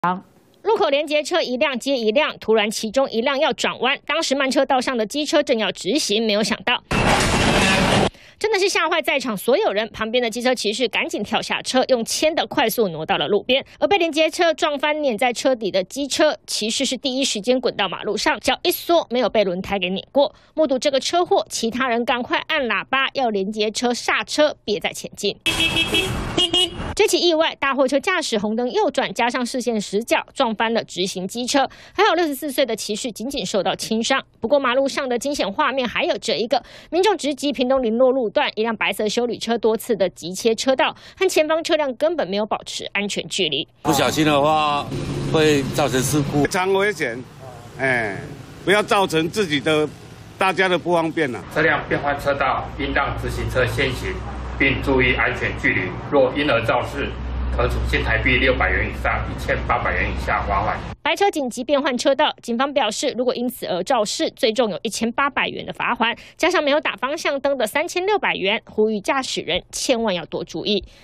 聯結車紅燈右轉撞機車 騎士急縮腳險遭輾│94看新聞
友邦連結區
◎ 訂閱【三立新聞網】最新影片搶先看